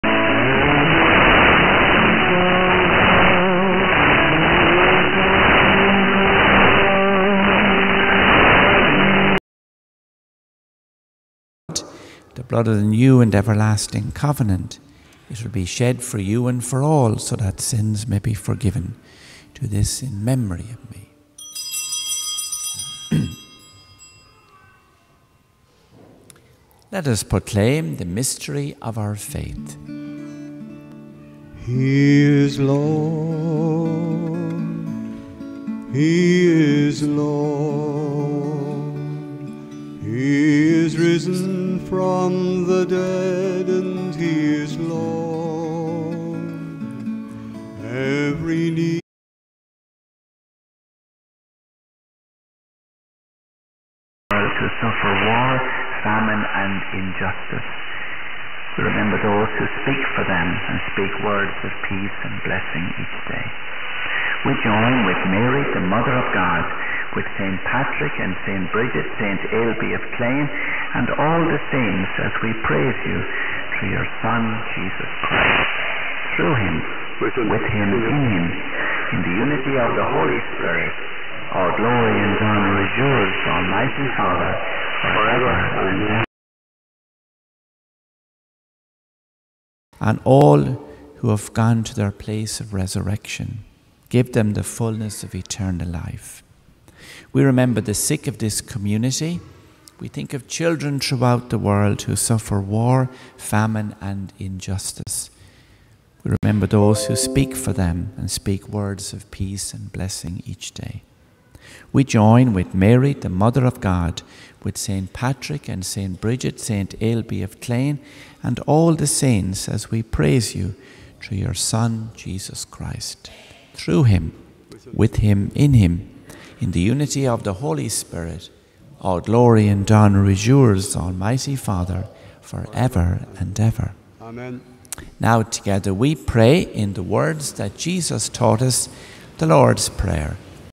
19.11.2023 09:55UTC, [WPAS], Sts.Patrick&Brigid Church, Clane, IRL, 27651.25kHz, 2814km, 10W, w/ID
Equipment: ELAD FDM S2 + dipole